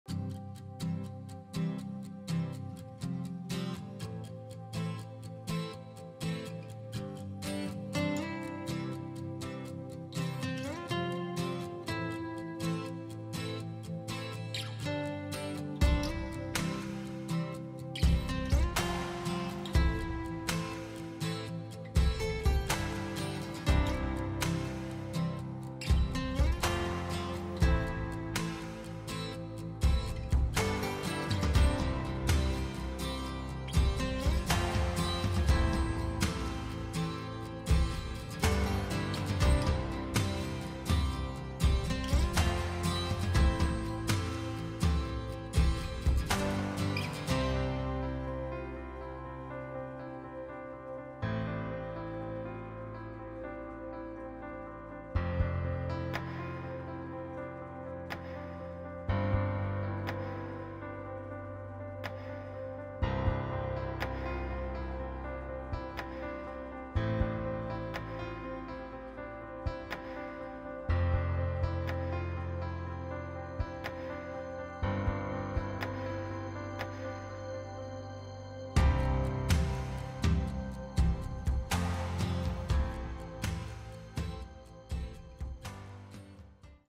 13218 Upper Applegate Road in Jacksonville, Oregon
🐟 Single-level home with over 650 feet of private Applegate River frontage - including a major steelhead spawning pool - located directly across from Sanctuary One, a 55-acre farm and animal refuge dedicated to environmental stewardship.